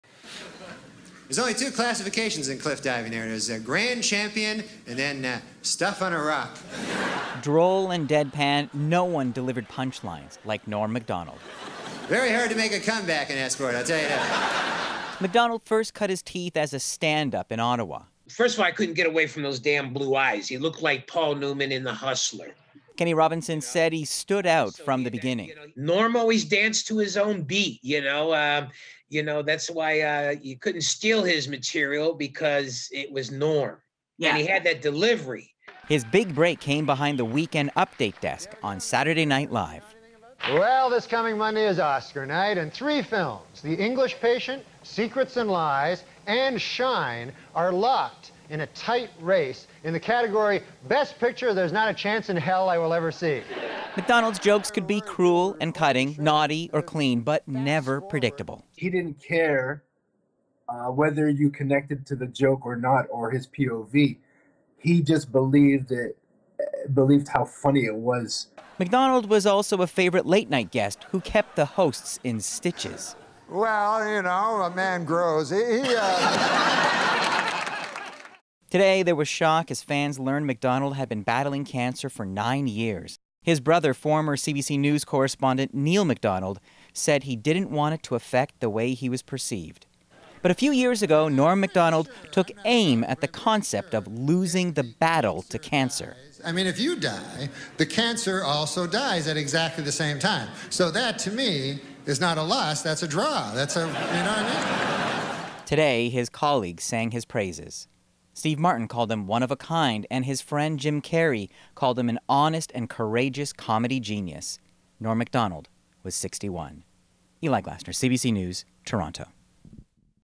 Canadian comedian Norm Macdonald dead at 61
Canadian-born comedian Norm Macdonald has died at 61 after a private battle with cancer. He is best known for hosting "Weekend Update" on Saturday Night Live.